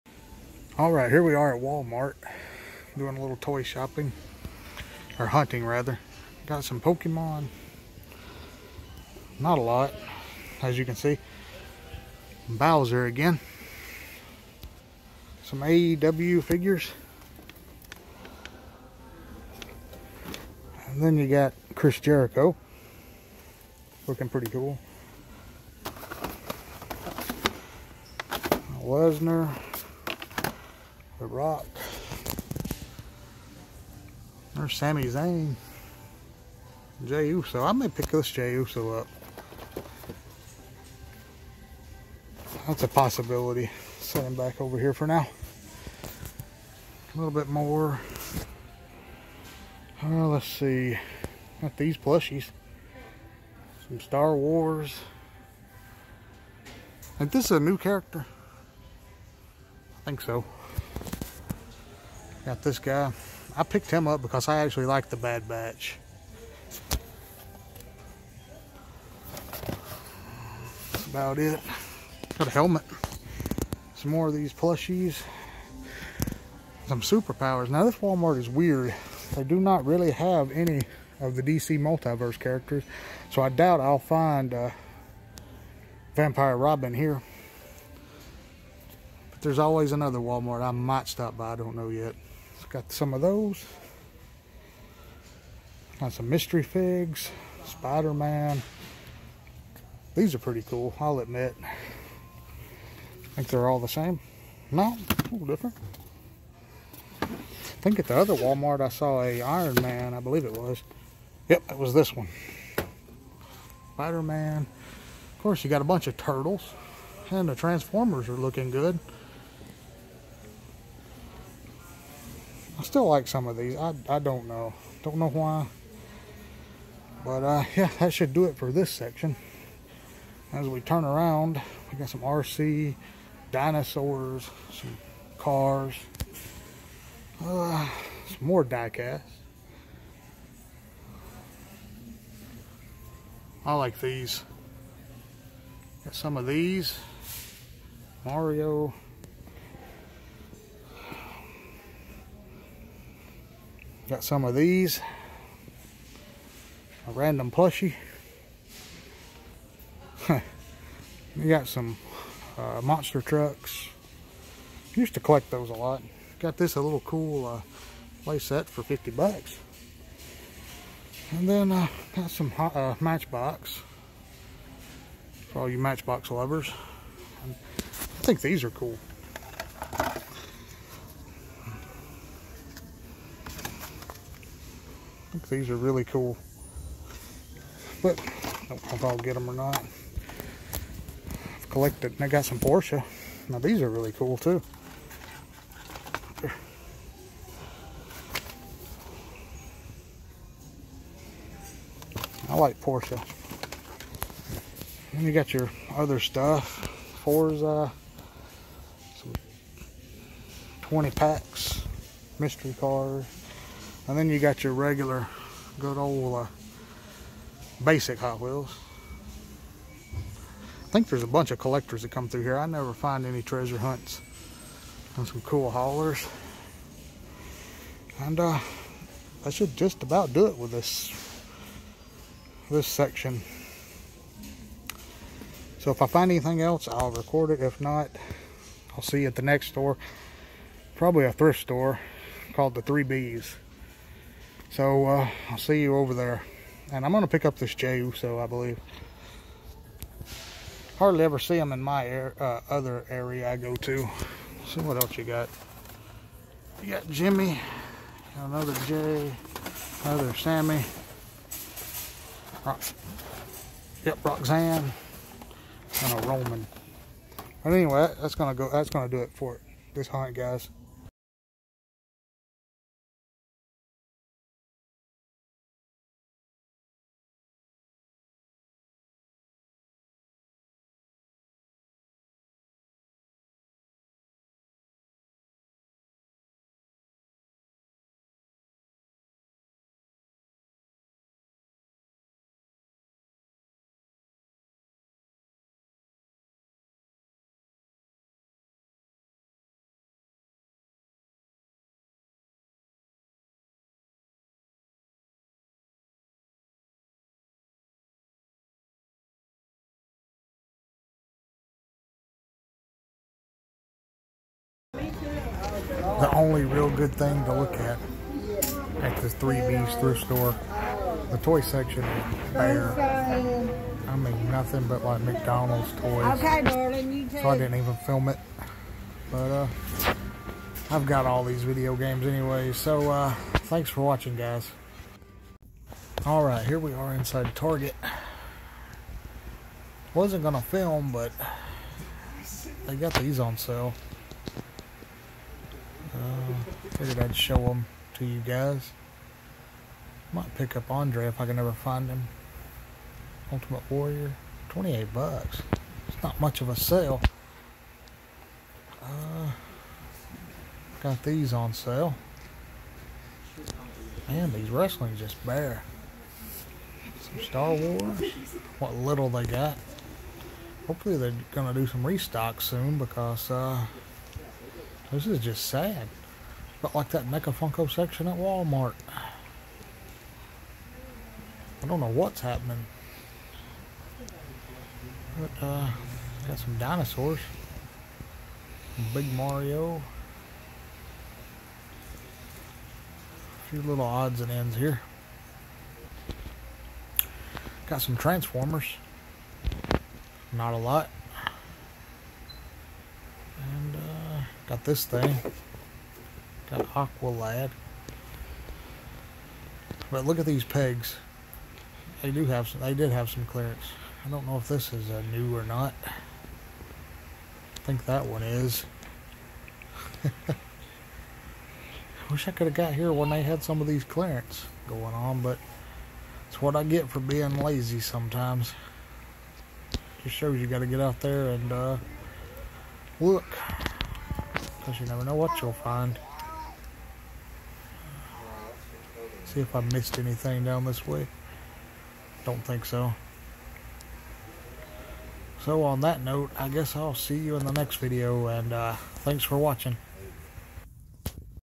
New WWE Figures Found | Walmart and Target Toy Hunt | Target Sales
Join me as I go toy hunting at Walmart and Target. New WWE figures found and Target had some sales.